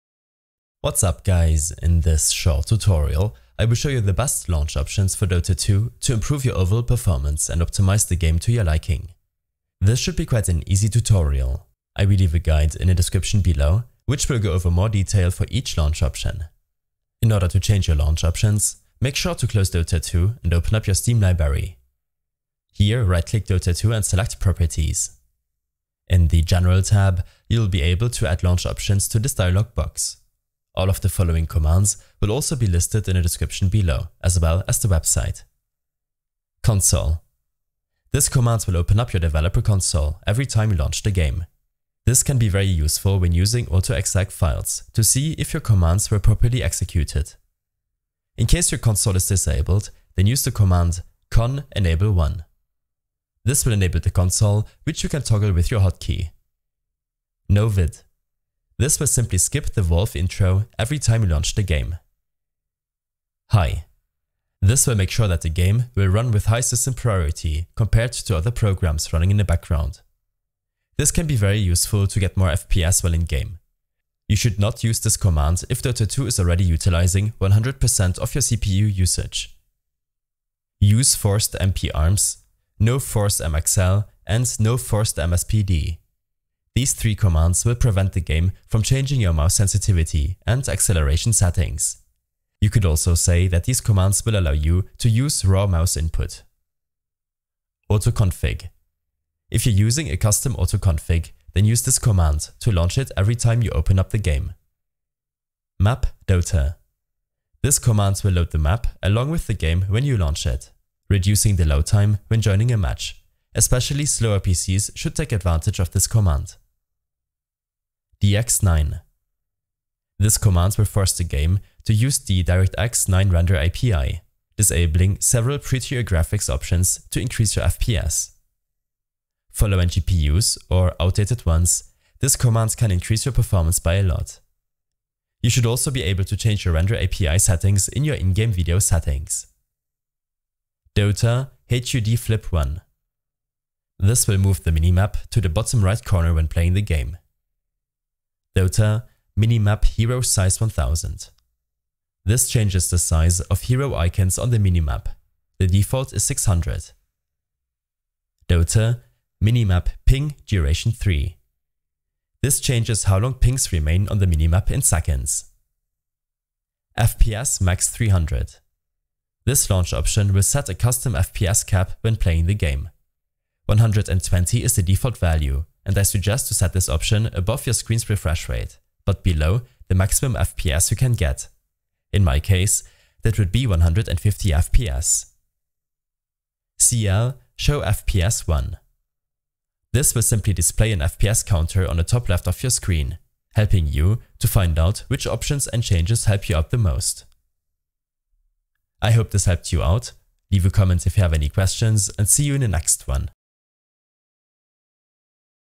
Dota 2 - Best Launch Options for Performance & Optimization
A short tutorial about best launch options for Dota 2 to improve your overall performance and optimize the game to your liking.